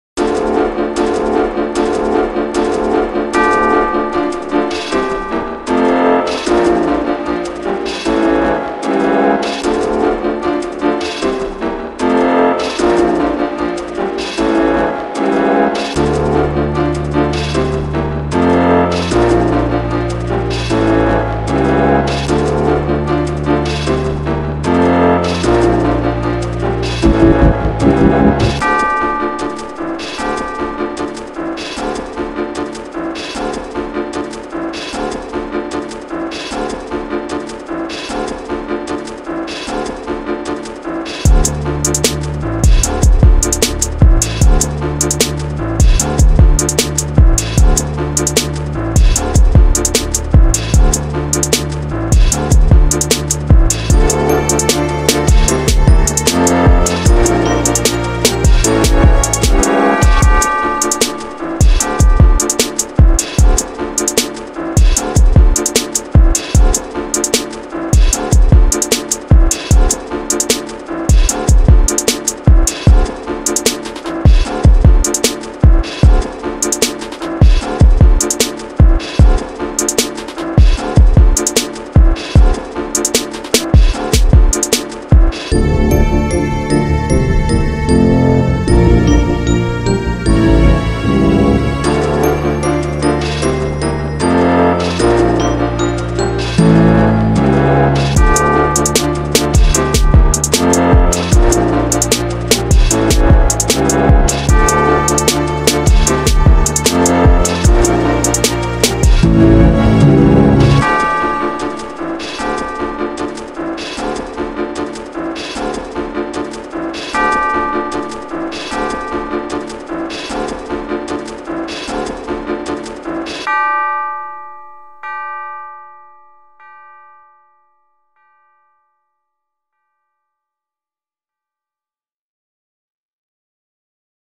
NEW! Spac3 L3agu3 2015 Sample
Finally back to producing! And here is a little something from Spac3 L3agu3's beat tape coming 2015.